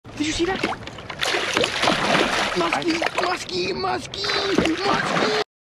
When you hook into a big muskie while bass fishing
when you hook into a big muskie while bass fishing! This is a short clip from a bass fishing video I did of us fishing for smallmouth bass in the winter with a jerkbait on the Susquehanna river! Its February and jerkbait season is in full swing here in Pennsylvania! watch the full video where I break down a few tips for fishing in cold water and fishing on river systems. I also go over how you should fishing a jerkbait in the winter! Hopefully you enjoy seeing this big tiger muskie! watch the full video to see the whole fight and see all the big smallmouth bass we catch fishing on the river in the winter!